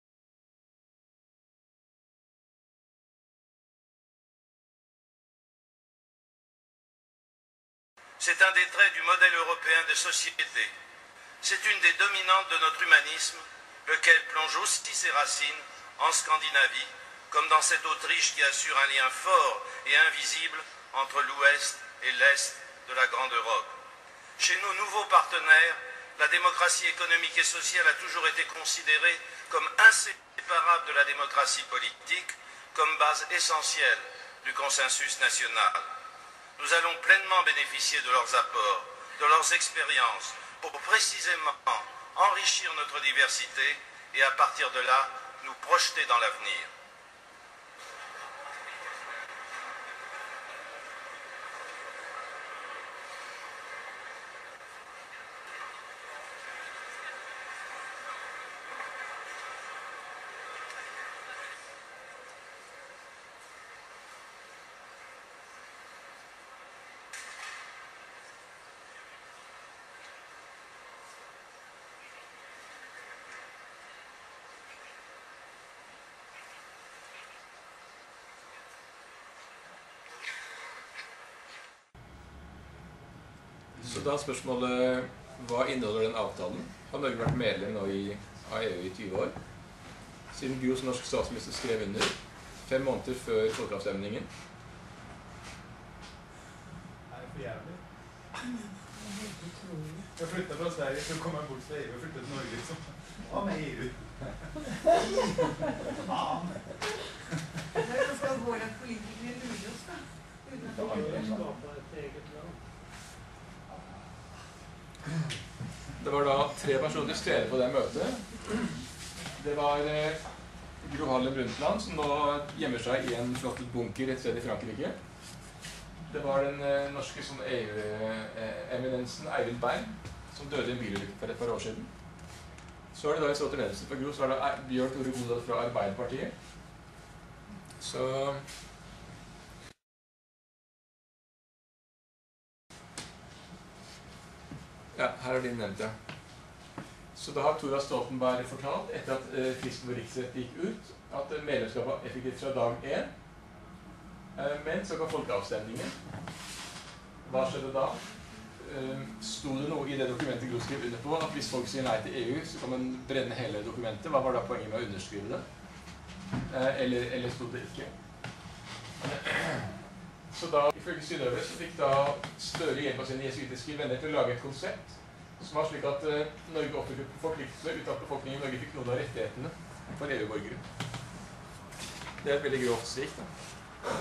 Gro Harlem Brundtland meldte Norge inn i EU 24 juni 1994
Video-bevis for at Gro Harlem Brundtland meldte Norge inn i EU 24 juni 1994, 5 måneder før folkeavstemningen om EU.